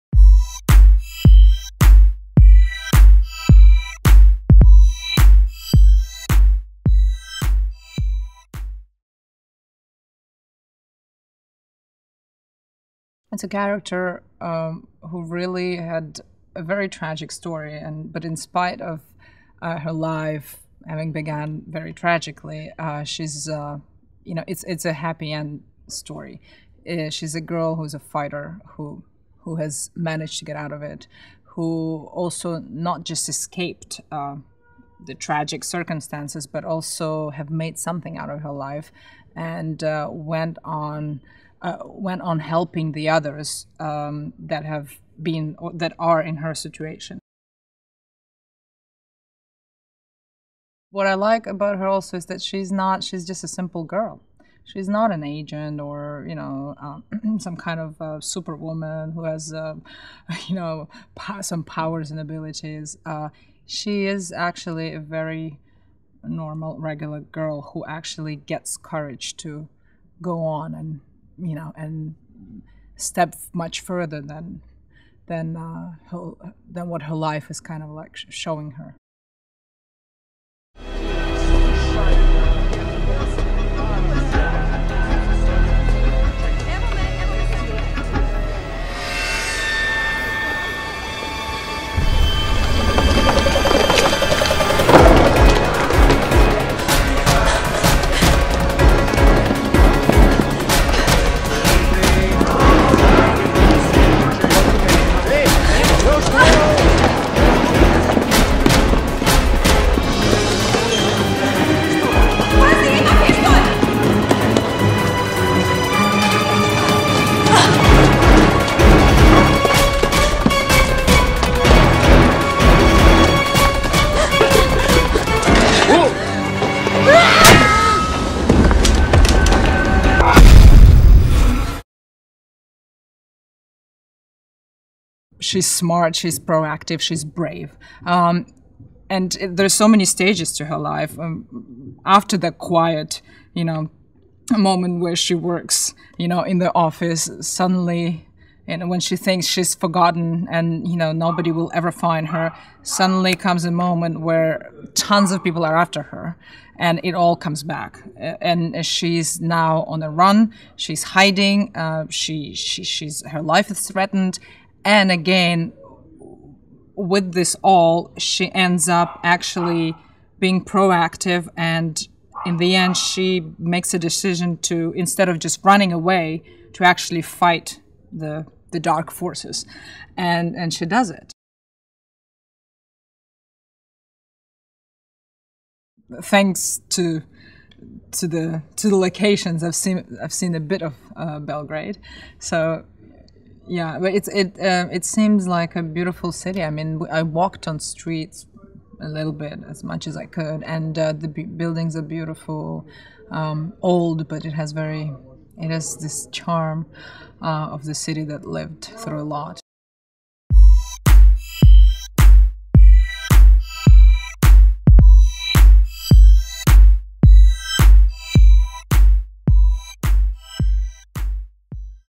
Olga Kurylenko Talks About Her New Film The November Man
Code named 'The November Man'; Peter Devereaux (Pierce Brosnan) is an extremely dangerous and highly trained ex-CIA agent, who is lured out of quiet retirement on a very personal mission.  He must protect valuable witness, Alice Fournier, (Olga Kurylenko) who could expose the truth behind a decades old conspiracy. He soon discovers this assignment makes him a target of his former friend and CIA protégé David Mason (Luke Bracey). With growing suspicions of a mole in the agency, there is no one Devereaux can trust, no rules and no holds barred.